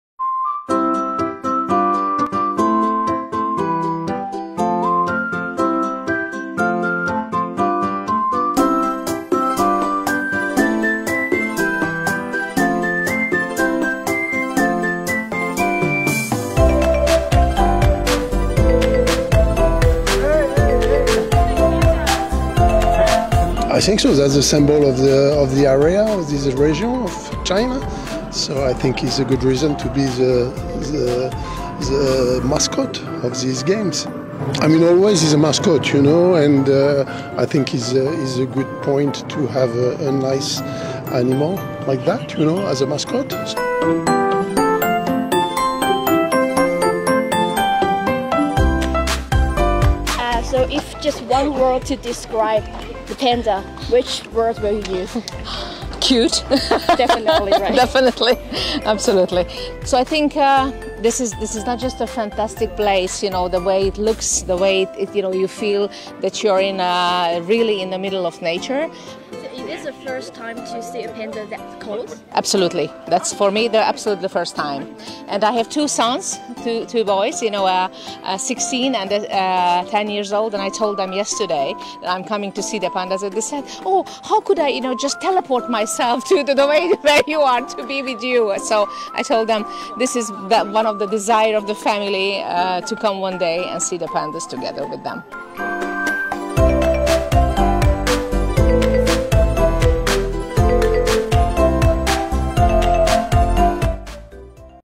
📸🐼 Media buzz alert!
📸🐼 Media buzz alert! The captivating appeal of these gentle giants has drawn media representatives from far and wide. Get ready to be charmed as you join them in experiencing the wonder of these adorable pandas. 🐾✨